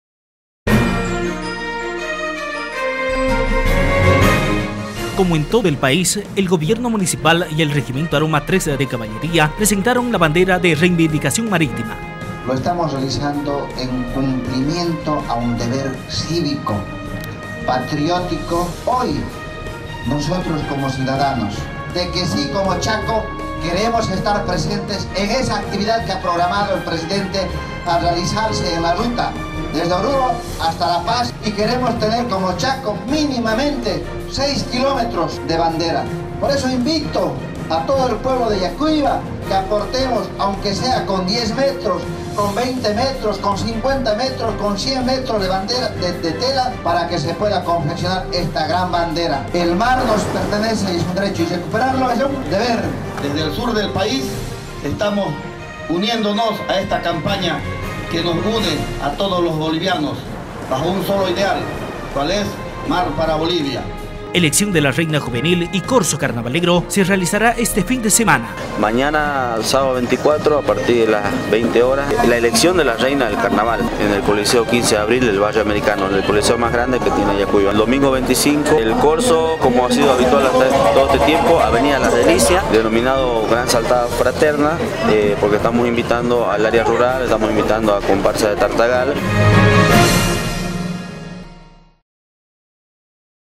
*Jornada Informativa viernes 23 de febrero de 2018*
*con 6 kilómetros de bandera, Yacuiba se suma a la Reivindicación Marítima de los Bolivianos.
*La alegría del carnaval del Lapacho se engalana con la elección de la Reina Juvenil.